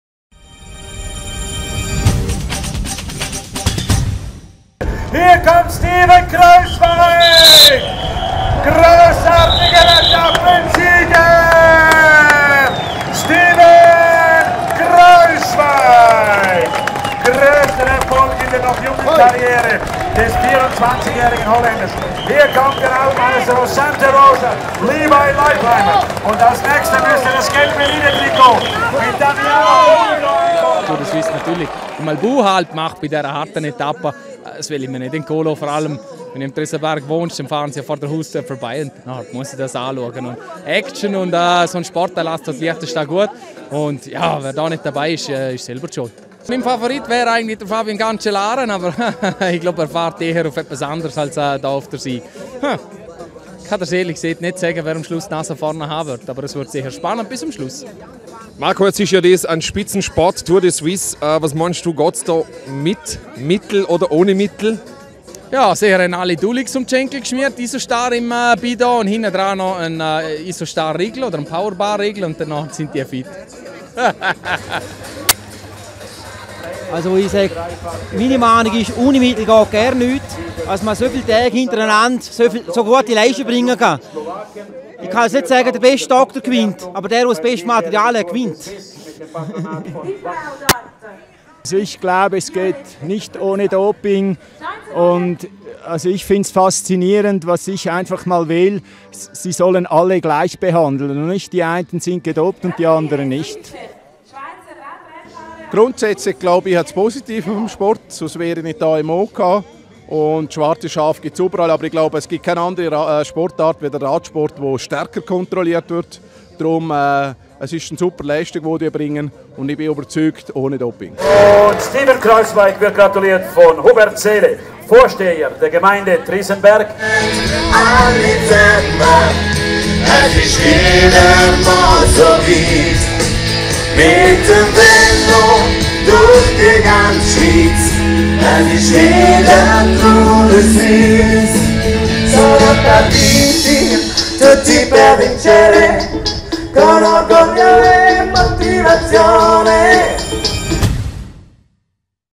TdS: Kruijswijk siegt in Malbun
MALBUN - Der Holländer Steven Kruijswijk gewann solo die 6. Etappe der Tour de Suisse in Malbun. Im Gesamtklassement verteidigte Damiano Cunego sein Leadertrikot.